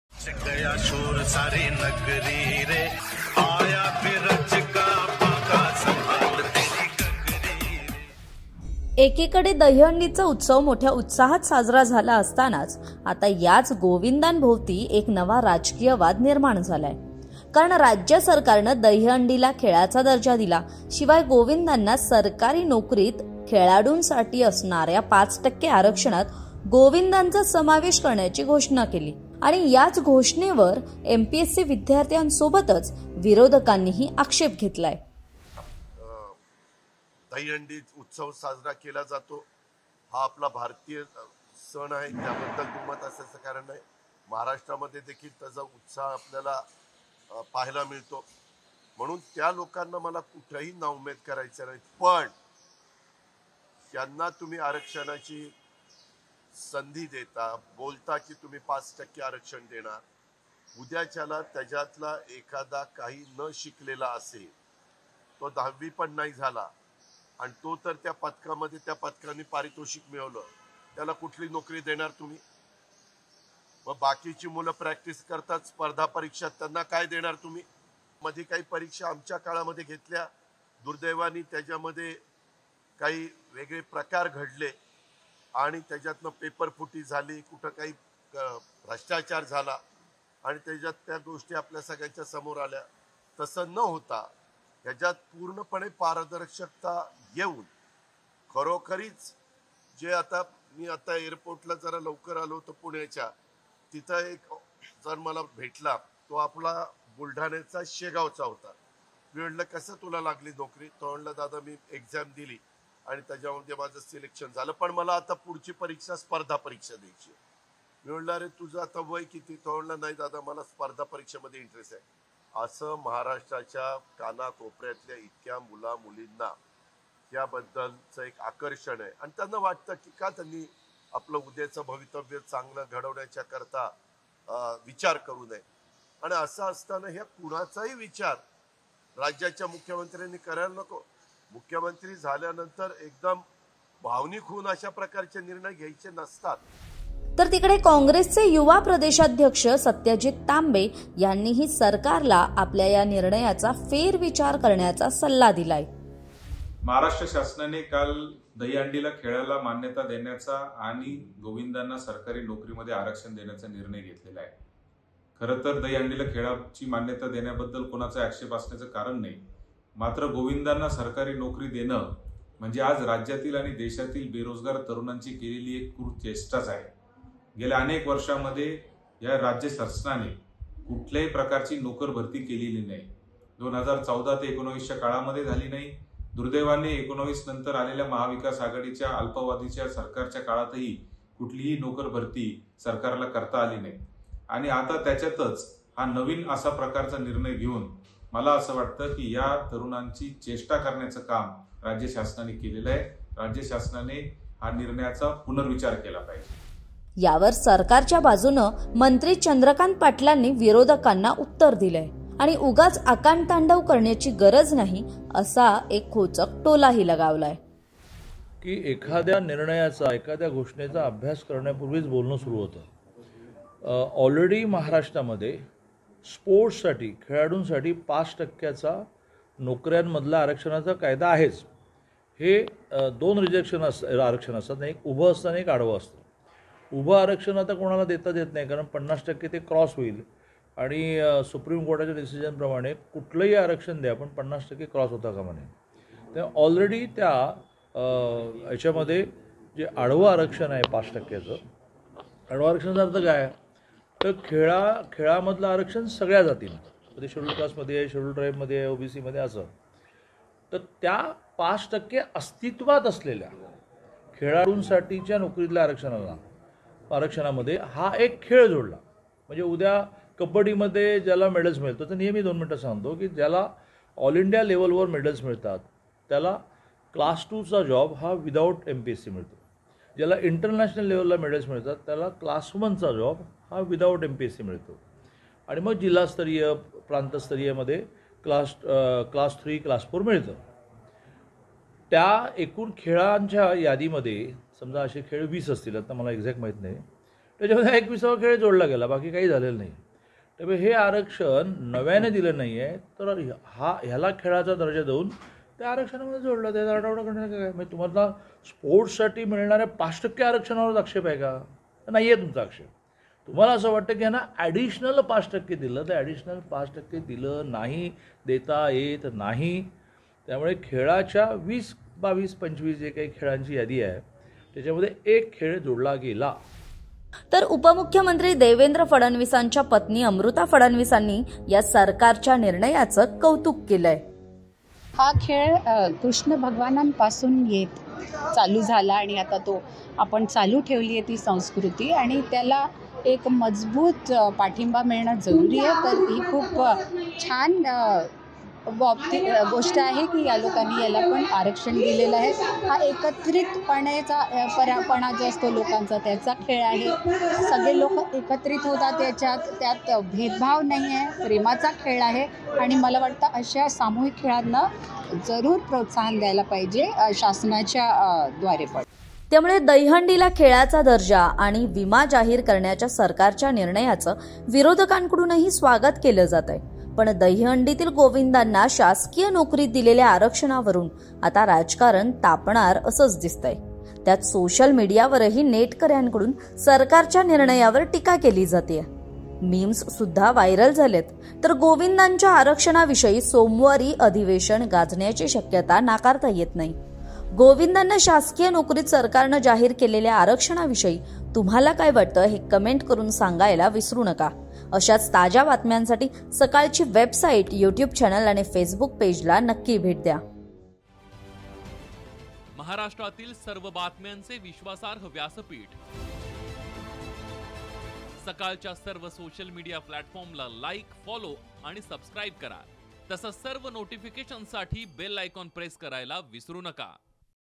Govinda Reservation in Government Service: गोविंदांना शासकीय नोकरीतील आरक्षणावरुन राजकारण तापलं
राज्यात २ वर्षांनी दहीहंडीचा उत्सव मोठ्या उत्साहात पार पडला. पण या उत्सवानंतर आज लगेच राजकारण तापायला सुरुवात झाली आहे. कारण शिंदे-फडणवीस सरकारनं गोविंदांना शासकीय नोकरीत आरक्षण जाहीर केलं. या निर्णयावर MPSC विद्यार्थ्यांप्रमाणेच विरोधकांनीही आक्षेप घेतला.